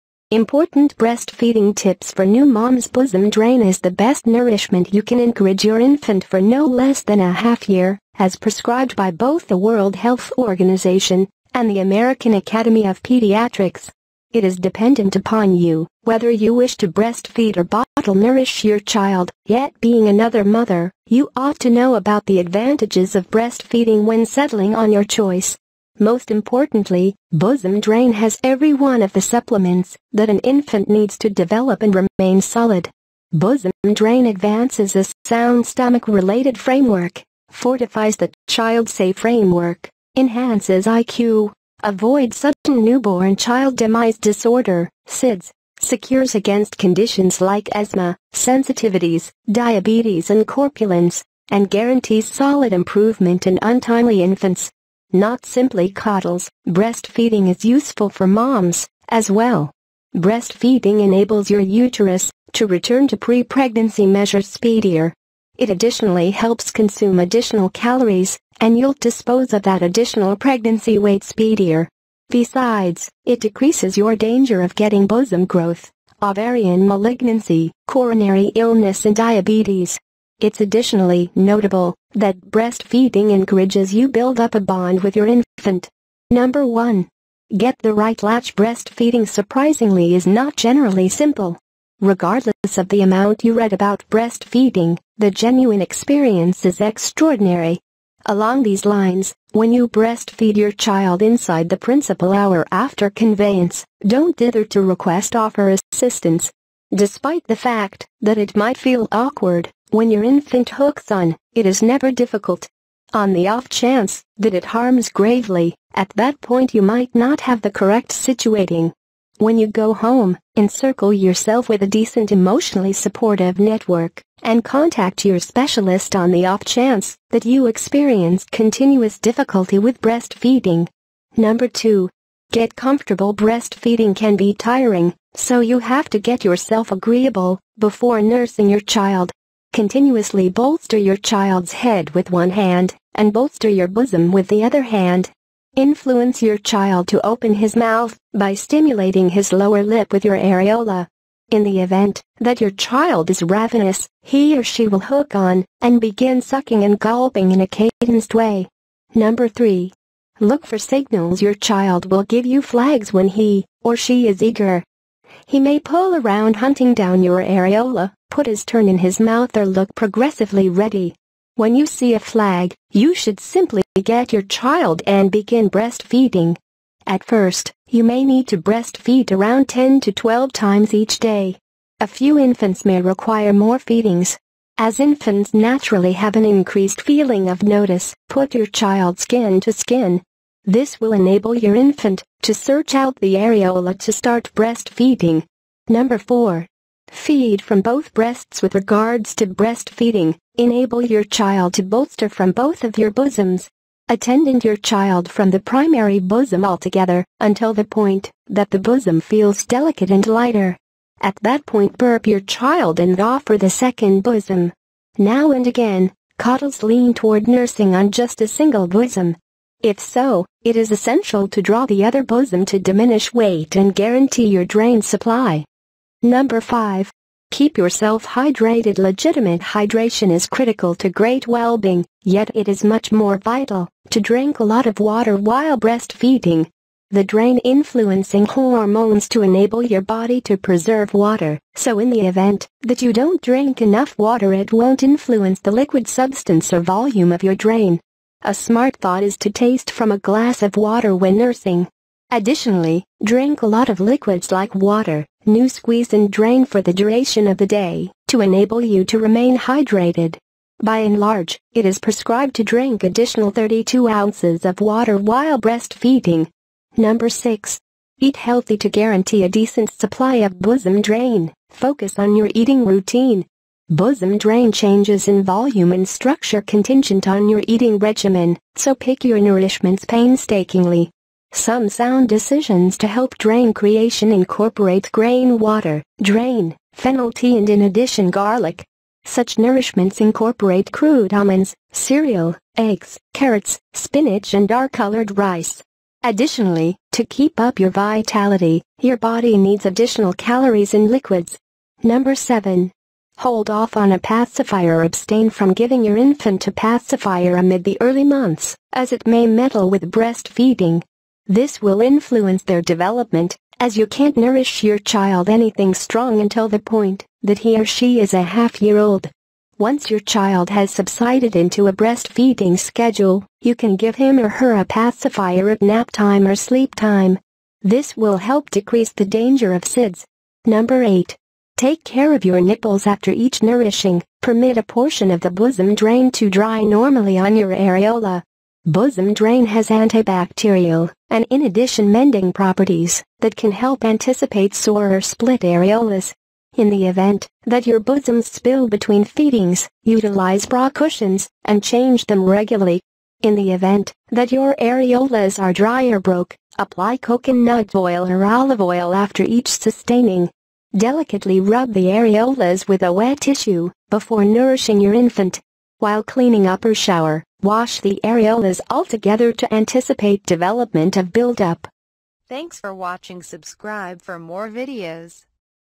Important Breastfeeding Tips for New Moms
Bosom drain is the best nourishment you can encourage your infant for no less than a

half year, as prescribed by both the World Health Organization and the American

Academy of Pediatrics.

It is dependent upon you whether you wish to breastfeed or bottle-nourish your

child, yet being another mother, you ought to know about the advantages of

breastfeeding when settling on your choice.

Most importantly, bosom drain has every one of the supplements that an infant needs

to develop and remain solid. Bosom drain advances a sound stomach related framework;

fortifies the child's safe framework; enhances IQ;

avoids sudden newborn child demise disorder (SIDS); secures against conditions like

asthma, sensitivities, diabetes and corpulence; and guarantees solid improvement in

untimely infants.

Not simply coddles, breastfeeding is useful for moms, as well. Breastfeeding enables

your uterus to return to pre-pregnancy measure speedier. It additionally helps

consume additional calories and you'll dispose of that additional pregnancy weight

speedier.

Besides, it decreases your danger of getting bosom growth, ovarian malignancy,

coronary illness and diabetes. It's additionally notable that breastfeeding

encourages you build up a bond with your infant.

#1. Get the Right Latch

Breastfeeding surprisingly is not generally simple. Regardless of the amount you

read about breastfeeding, the genuine experience is extraordinary. Along these

lines, when you breastfeed your child inside the principal hour after conveyance,

don't dither to request offer assistance.

Despite the fact that it might feel awkward when your infant hooks on, it is never

difficult. On the off chance that it harms gravely, at that point you might not have

the correct situating.

When you go home, encircle yourself with a decent emotionally supportive network and

contact your specialist on the off chance that you experience continuous difficulty

with breastfeeding.

#2. Get Comfortable

Breastfeeding can be tiring, so you have to get yourself agreeable before nursing

your child. Continuously bolster your child's head with one hand and bolster your

bosom with the other hand.

Influence your child to open his mouth by stimulating his lower lip with your

areola. In the event that your child is ravenous, he or she will hook on and begin

sucking and gulping in a cadenced way.

#3. Look for Signals

Your child will give you flags when he or she is eager. He may pull around hunting

down your areola, put his turn in his mouth or look progressively ready. When you

see a flag, you should simply get your child and begin breastfeeding.

At first, you may need to breastfeed around 10 to 12 times each day.

#4. Feed from Both Breasts

With regards to breastfeeding, enable your child to bolster from both of your

bosoms. Attendant your child from the primary bosom altogether, until the point that

the bosom feels delicate and lighter. At that point burp your child and offer the

second bosom.

#5. Keep Yourself Hydrated

Legitimate hydration is critical to great wellbeing, yet it is much more vital to

drink a lot of water while breastfeeding. The drain influencing hormones to enable

your body to preserve water, so in the event that you don't drink enough water it

won't influence the liquid substance or volume of your drain.

#6. Eat Healthy

To guarantee a decent supply of bosom drain, focus on your eating routine. Bosom

drain changes in volume and structure contingent on your eating regimen, so pick

your nourishments painstakingly.

Some sound decisions to help drain creation incorporate grain water, drain, fennel

tea and in addition garlic.

Such nourishments incorporate crude almonds, cereal, eggs, carrots, spinach and dark

colored rice. Additionally, to keep up your vitality, your body needs additional

calories and liquids.

#7. Hold Off on a Pacifier

Abstain from giving your infant a pacifier amid the early months, as it may meddle

with breastfeeding. This will influence their development, as you can't nourish your

child anything strong until the point that he or she is a half year old.

Once your child has subsided into a breastfeeding schedule, you can give him or her

a pacifier at naptime or sleep time. This will help decrease the danger of SIDS.

#8. Take Care of Your Nipples

After each nourishing, permit a portion of the bosom drain to dry normally on your

areola. Bosom drain has antibacterial and in addition mending properties that can

help anticipate sore or split areolas.

buildup.